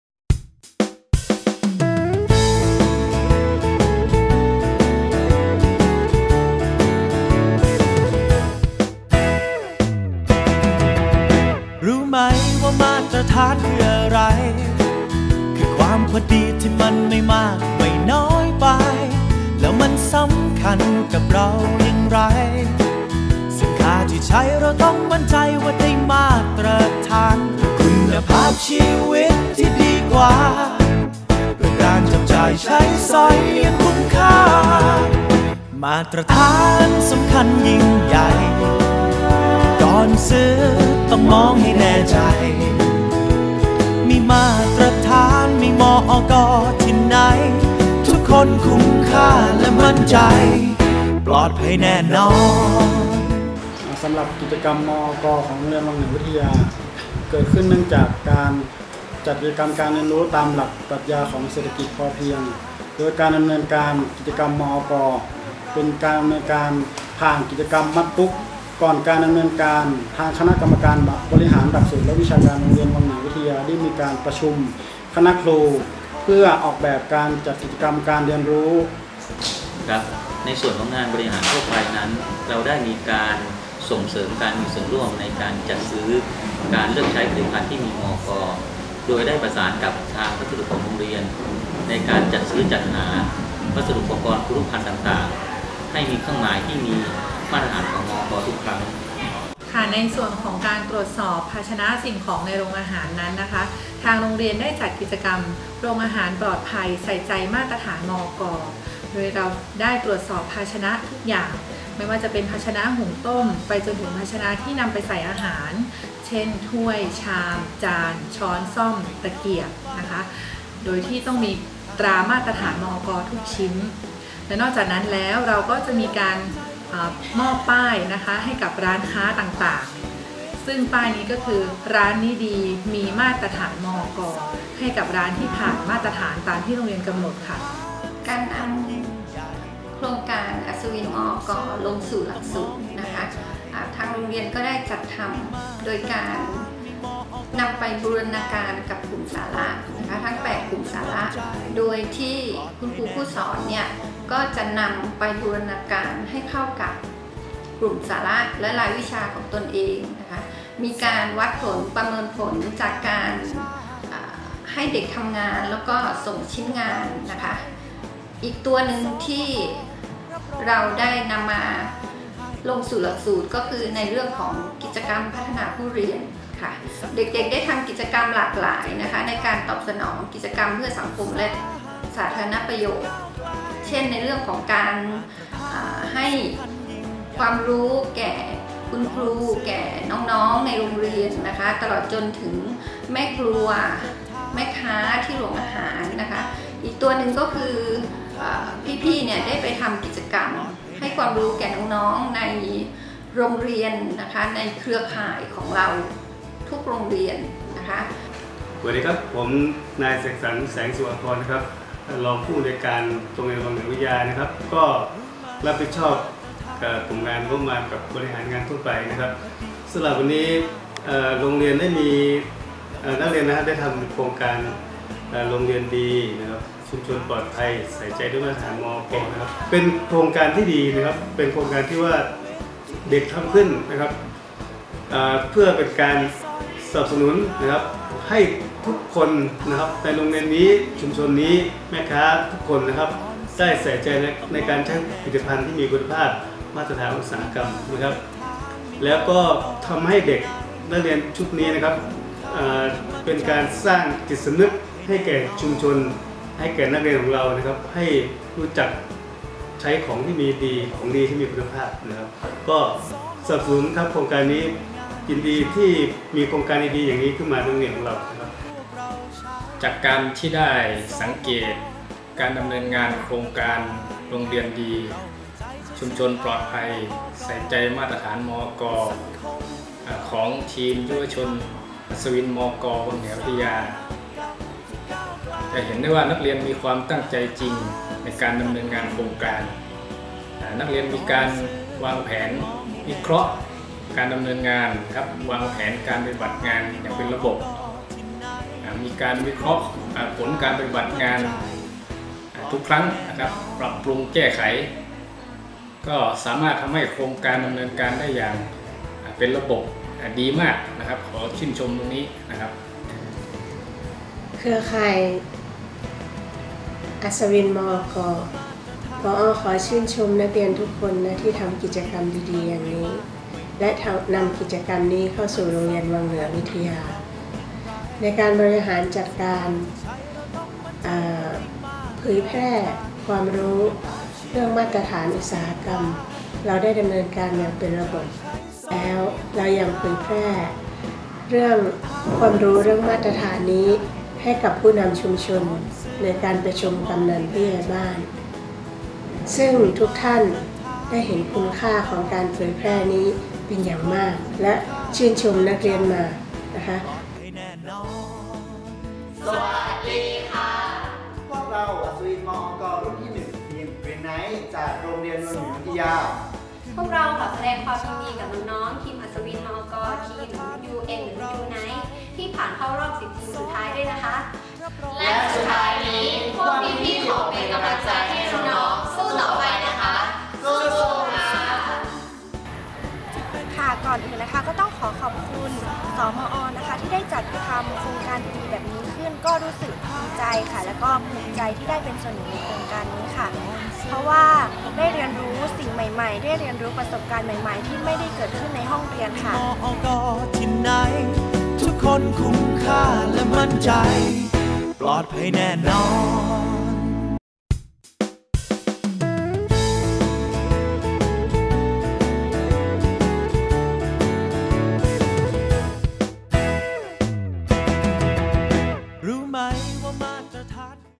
อัศวิน มอก.วังเหนือวิทยา
โรงเรียนวังเหนือวิทยา อ.วังเหนือ จ.ลำปาง สพม.35 ได้รับการประเมินโครงการโรงเรียนดีชุมชนปลอดภัยใส่ใจด้วยมาตรฐาน มอก. วันที่ 7 พฤศจิกายน 2559